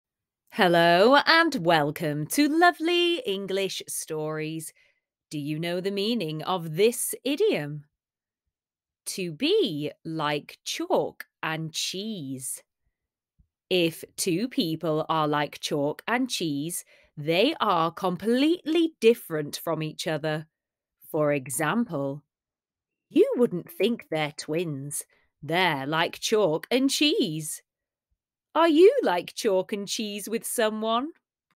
ADVANCED ENGLISH IDIOM | To be like chalk and cheese | Lovely English Stories
✨ Want to learn more British English vocabulary? Check out these 30 British English words with their meanings and explanations

In this for learningenglish we explain what the British English idiom ''to be like chalk and cheese" means.

learn english idioms, popular english idioms, chalk and cheese meaning,